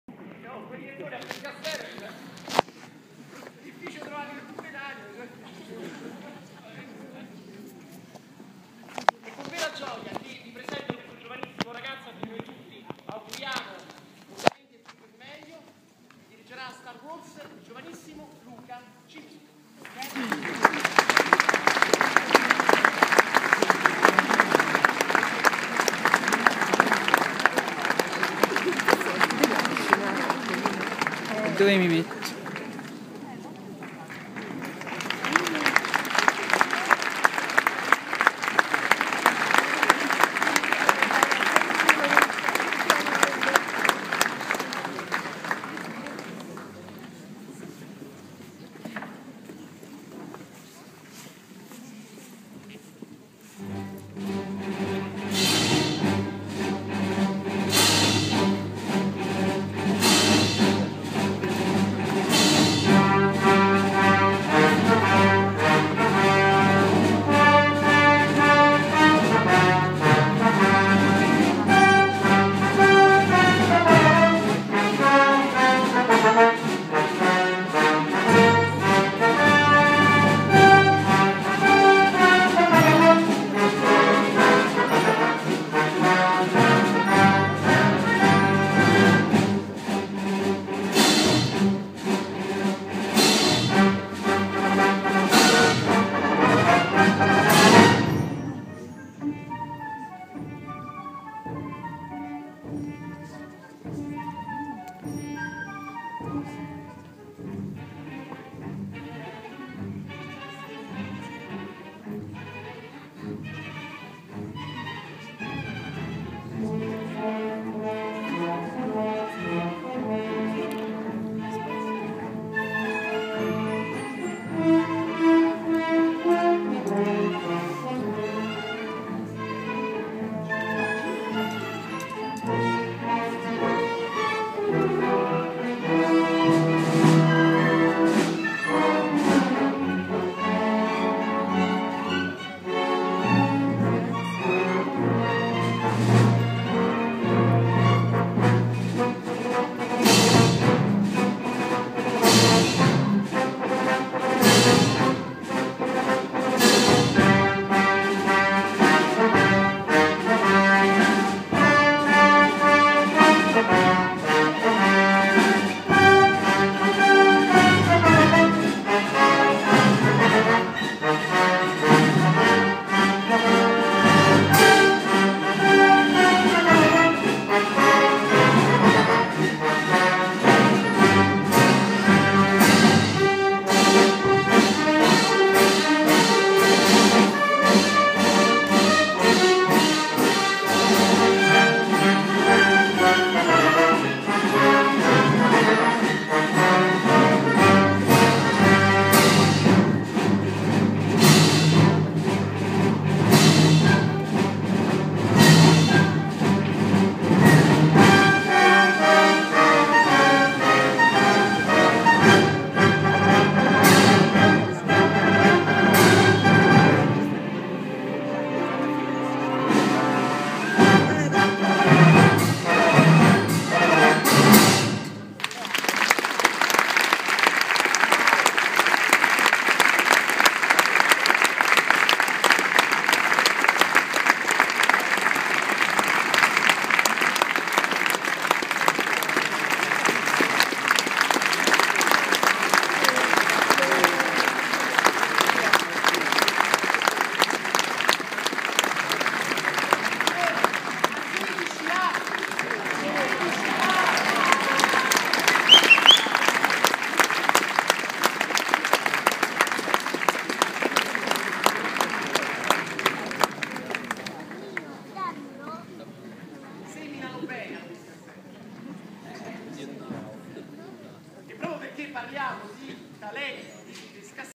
Luca Cimini direttore d'Orchestra a Pescasseroli
Luca Cimini ha affiancato il M. Jacopo Sipari di Pescasseroli nella direzione dell'Orchestra Marco dall'Aquila a Pescasseroli